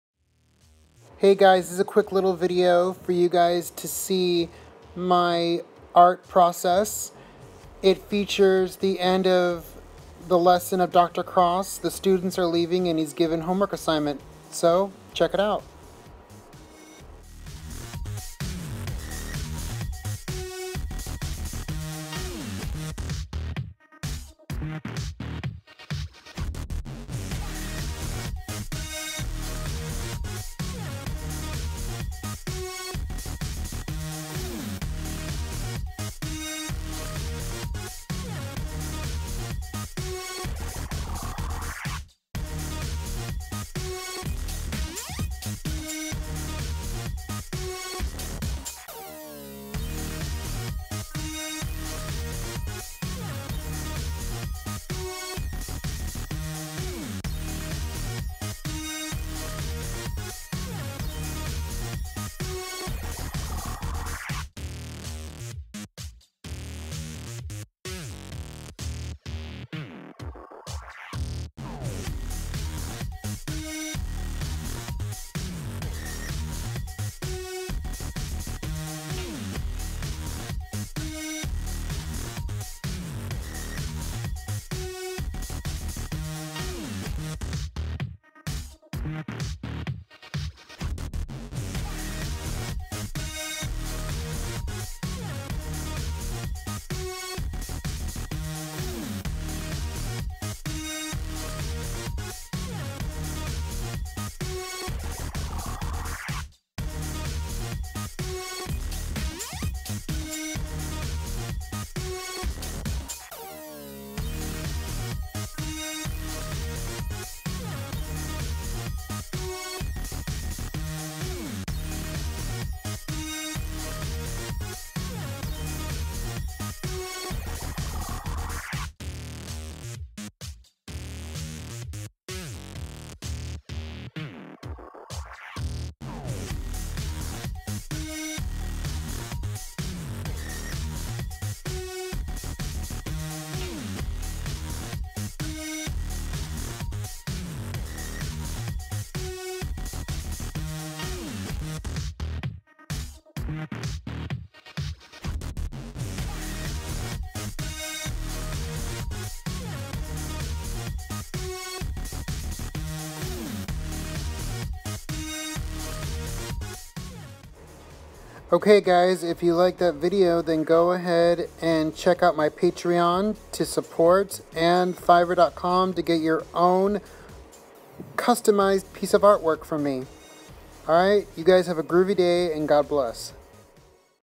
Jimani's Art: a quicky
this is a quick time laps video one of my recent art work i just finished yesterday=8/28/2018 for my comic The In-Siderz featuring the end of Dr. John Cross JR class lesson the students leaving and Dr. Cross givng out the homework assignments